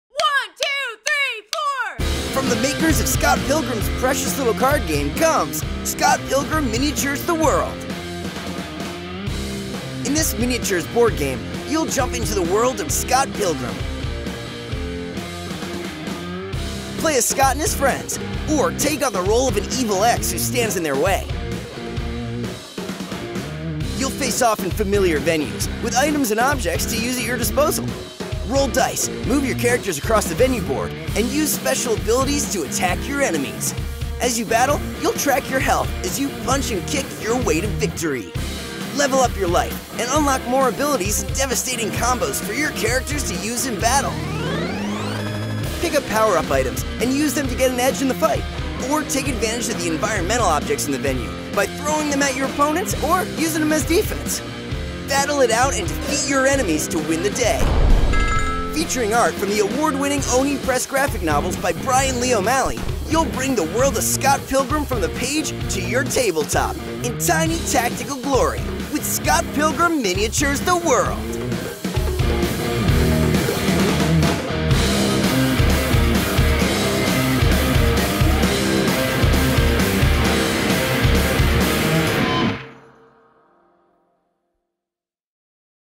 Scott Pilgrim Miniatures The World: Official Kickstarter Trailer
Scott Pilgrim Miniatures The World, designed by Erica Bouyouris, and based on the Oni Press graphic novel series Scott Pilgrim, by Bryan Lee O'Malley, you’ll jump onto the tabletop to play as Scott and his friends or take on the role of an evil ex who stands in their way.

Follow Renegade Game Studios for updates, photos and more exciting information on Scott Pilgrim Miniatures the World and other exciting games!